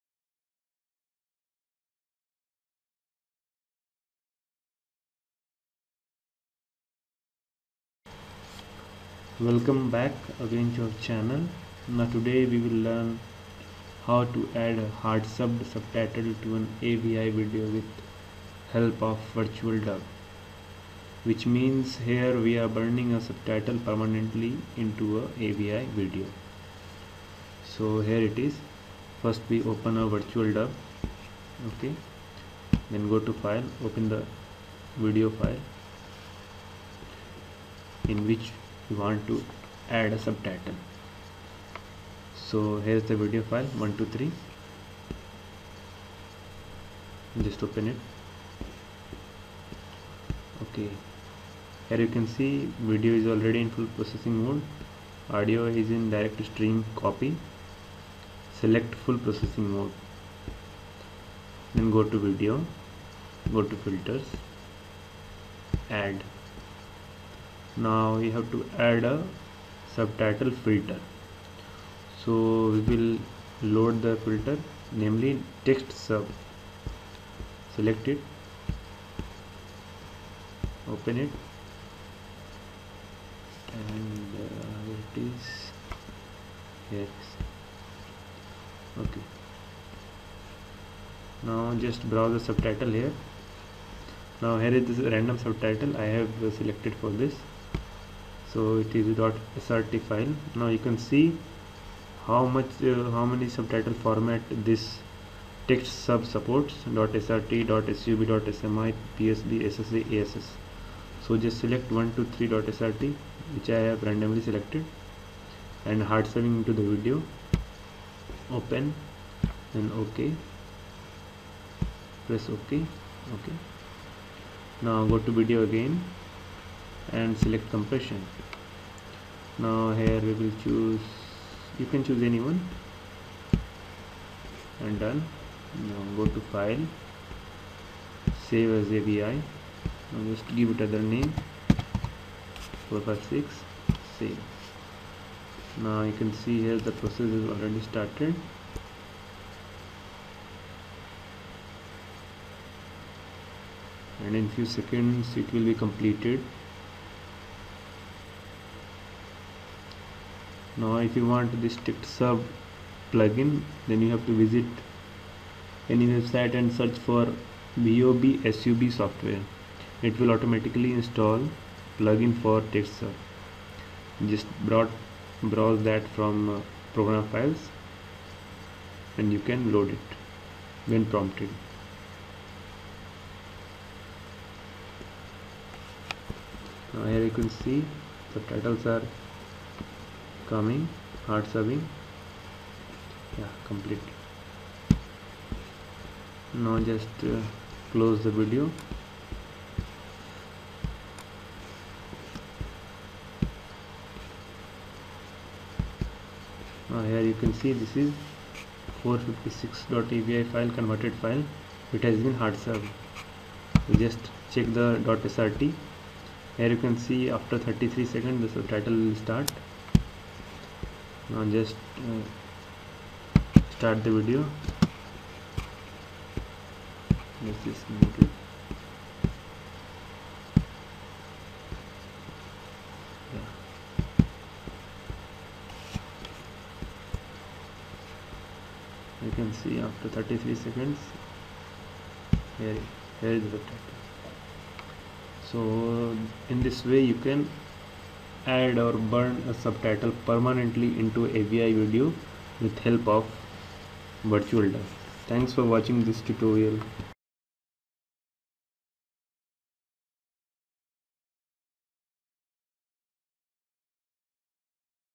How to add hard subtitle to avi video permanently with virtualdub
Here I have made a small video tutorial for explaining the method to add a hardsubbed subtitle to an .avi video using virtual dub.
Softwares needed - Virtualdub and textsub plugin for this

Textsub plugin - Download vobsub and install it and textsub plugin comes with this package.